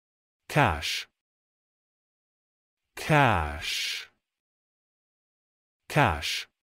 How to Pronounce: Cache
Noun: a hiding place, especially for valuables, food, or ammunition.  In terms of computers, a cache is temporary storage for quick access.

IPA:  kæʃ
1 Syllables: KASH
*Pronounced exactly the same as "cash"

Rhymes with: ash bash, cash, dash, gash, hash, lash, mash, gnash, stash, mustache, clash